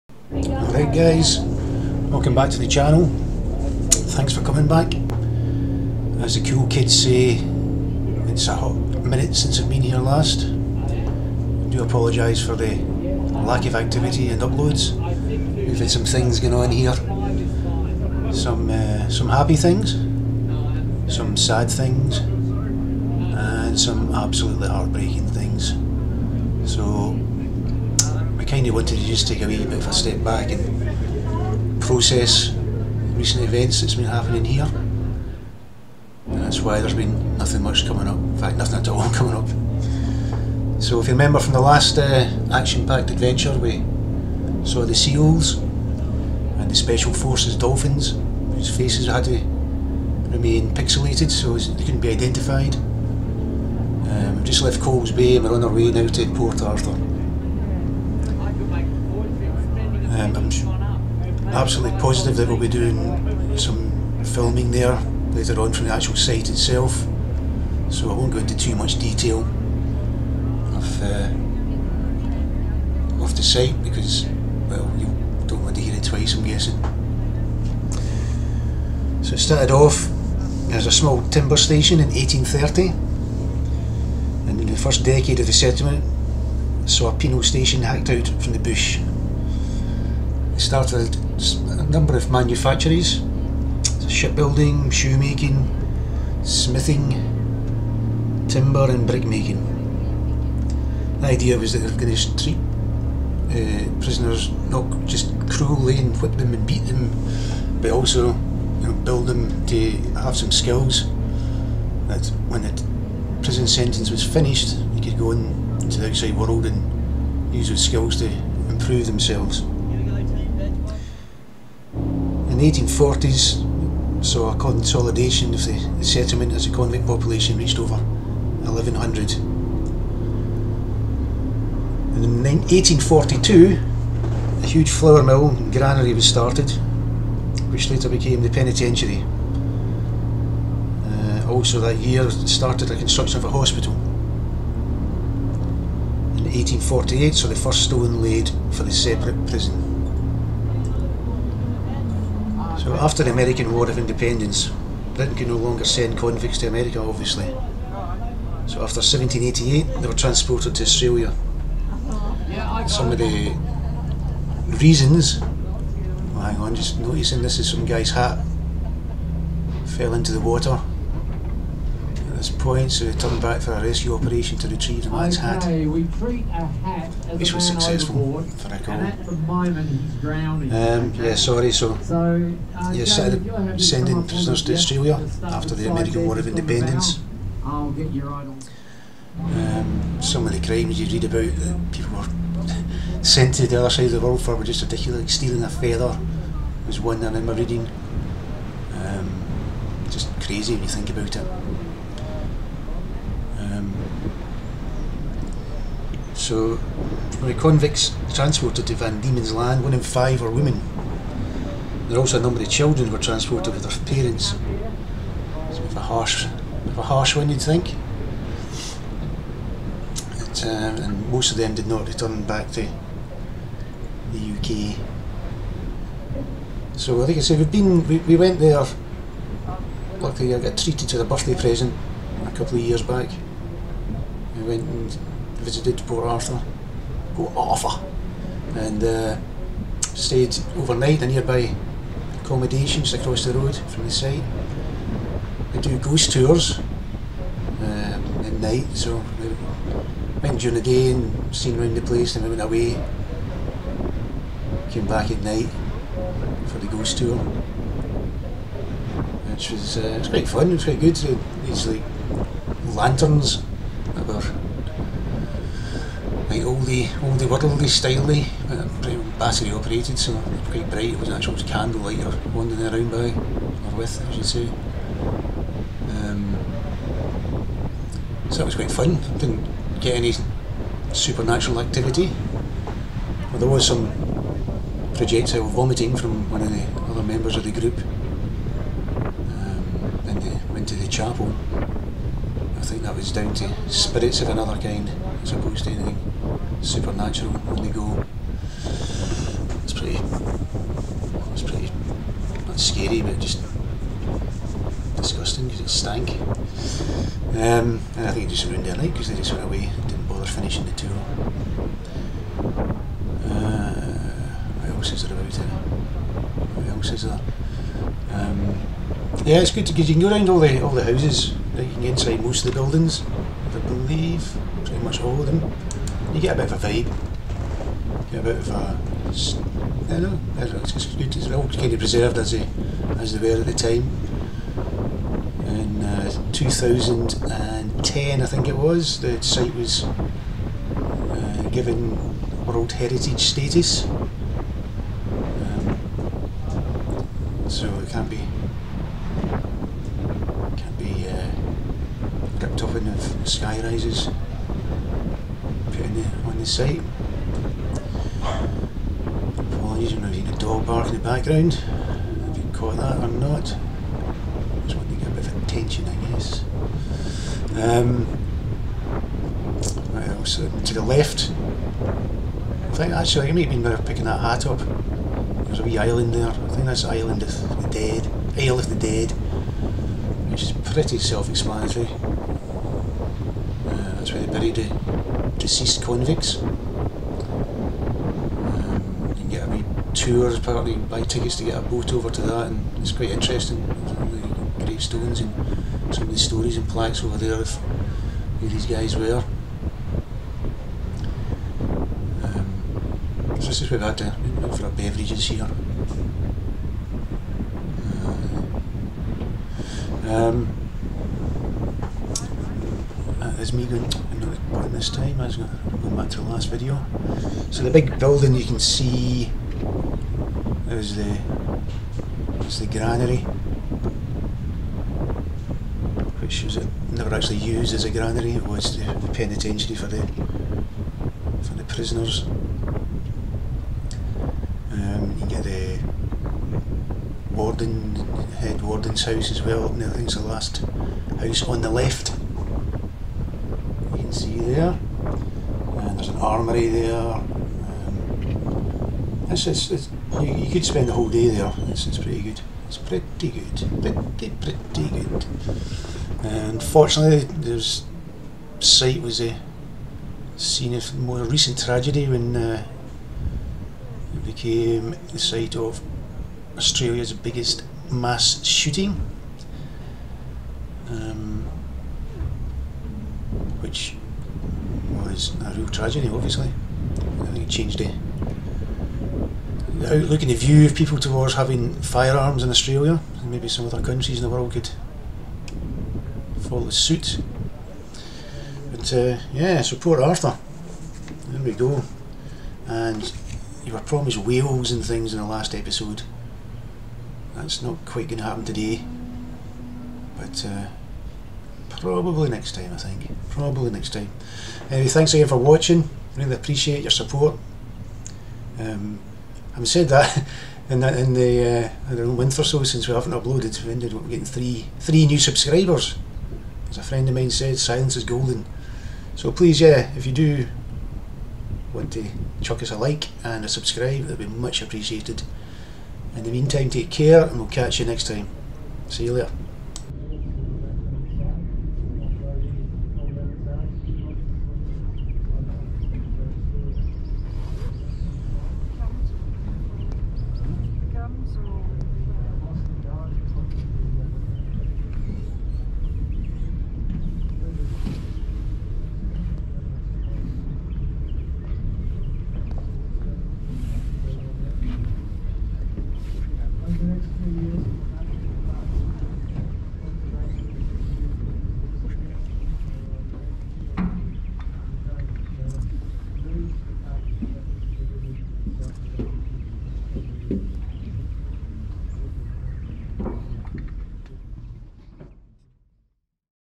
Onwards to Port Arthur
We now head to the outskirts of Port Arthur.